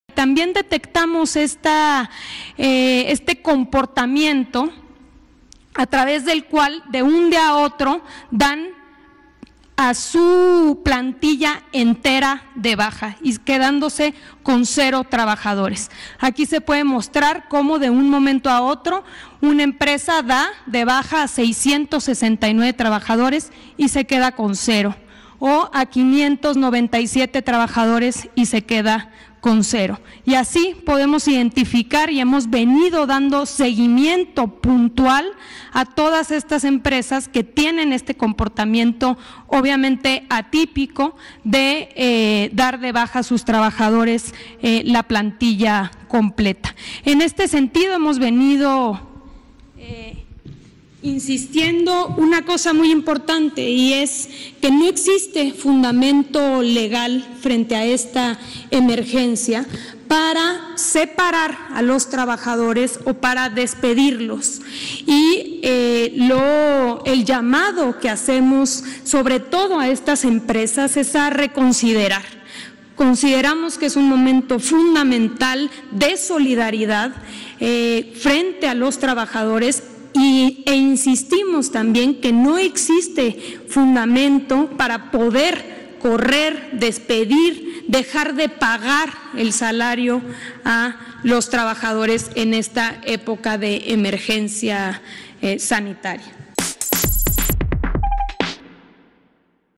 No existe fundamento legal para despedir trabajadores frente a contingencia: STPS
«En México se han registrado empresas con hasta 669 trabajadores que han despedido al 100% de su personal debido a la crisis del coronavirus», informó la titular de la STPS, Luisa Alcalde.